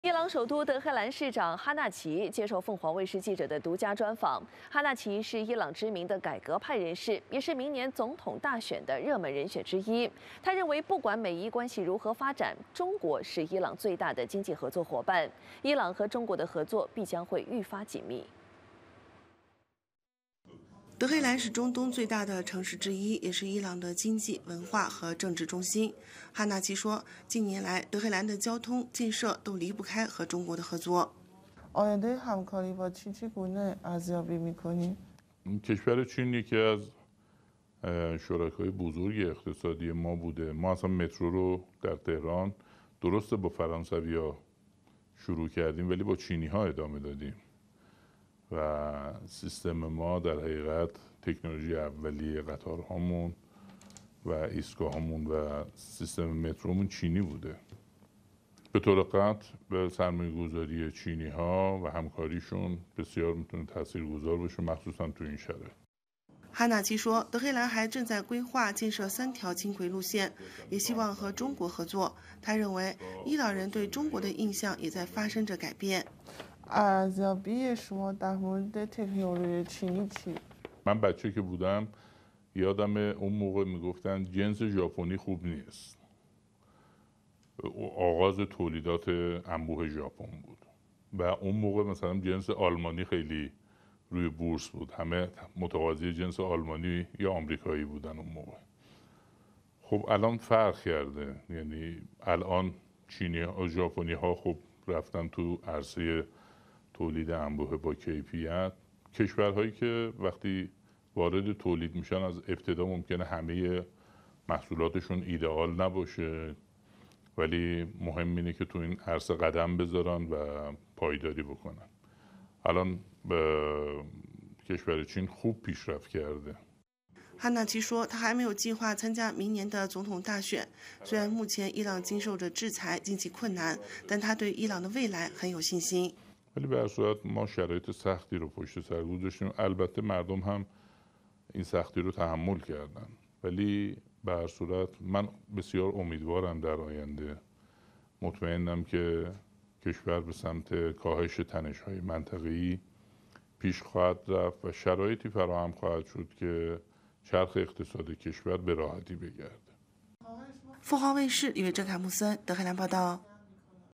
獨家｜德黑蘭市長：伊朗中國合作必將越緊密 20201110【下載鳳凰秀App，發現更多精彩】
伊朗首都德黑蘭市長哈納齊7日接受鳳凰記者的獨家專訪，哈納齊是伊朗知名的改革派人士也是明年總統大選的熱門人選之一。他如何看待與中國未來的合作？